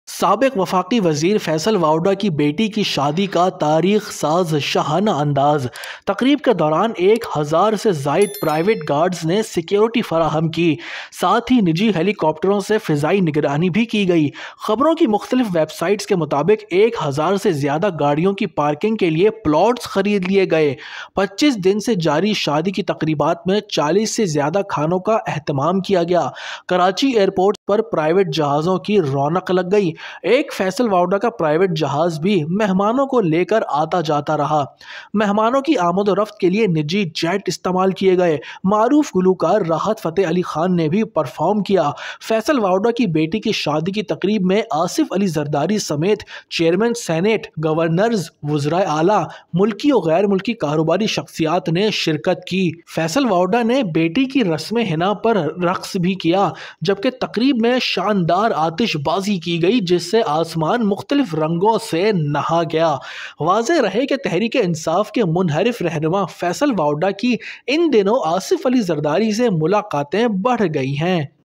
Faisal Vawda Ne Beti Ki Shadi Mein Kitne Arab Rupe Laga Diya??
Dekhiye Faisal Vawda ki beti ki shaadi se judi sabse taaza khabar! Is video mein, hum lekar aaye hain exclusive details aur shadi ki mithas. Janiye kya hain taiyaariyan aur kaise manai gayi yeh khaas moqa. 🤵👰